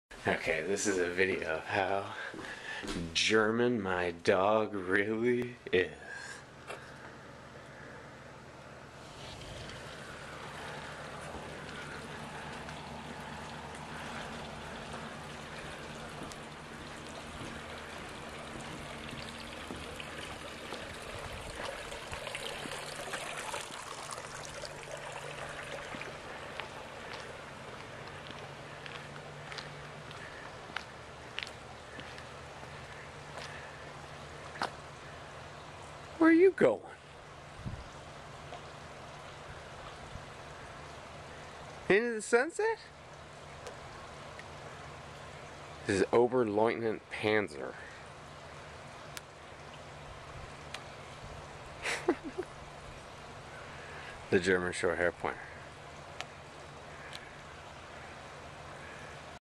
Panzer - German Shorthaired Pointer
He is my first dog & he is a great dog!!
Steady as you could ever need on land & sea.
What this dog does he does for me & I love him for it!
German as a dog could ever be he takes orders & doesn't care.
It is what I said to do, so he does it.
My good boy, Panzer.